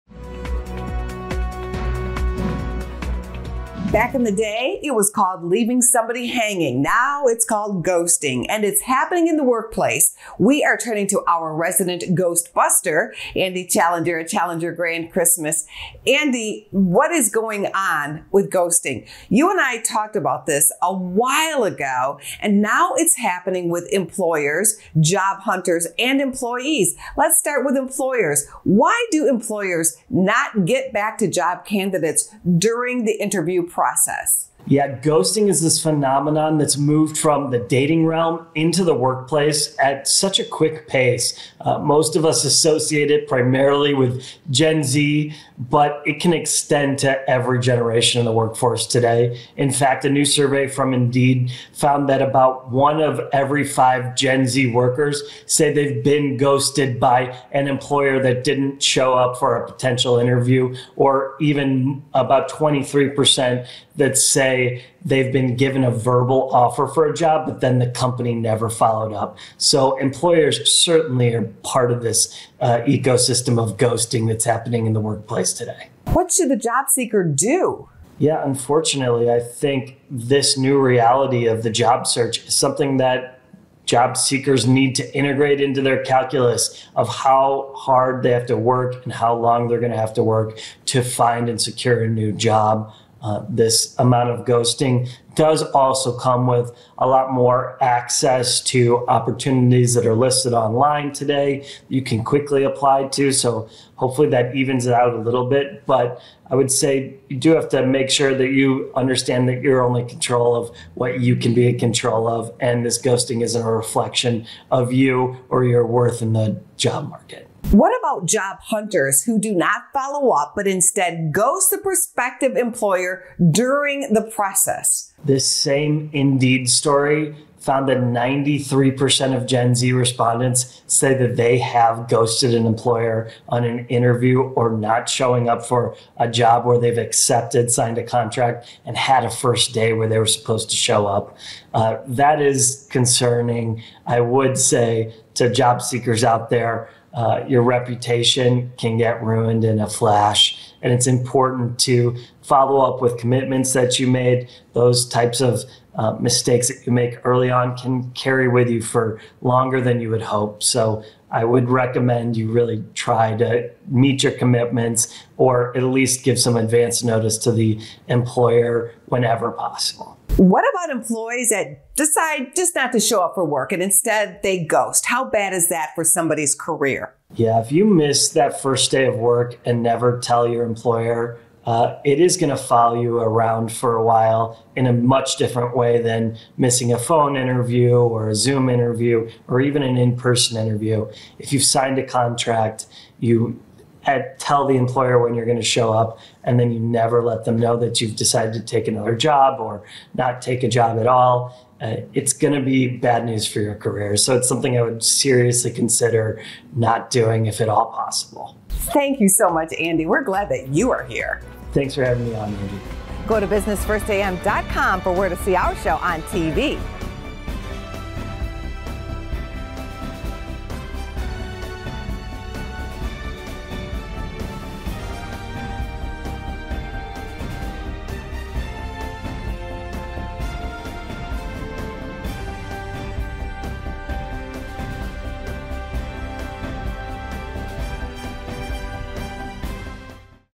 What To Do If You Are 'GHOSTED' In Job Hunting
Andrew Challenger, SVP, Head of Sales and Media at Challenger, Gray & Christmas, Inc.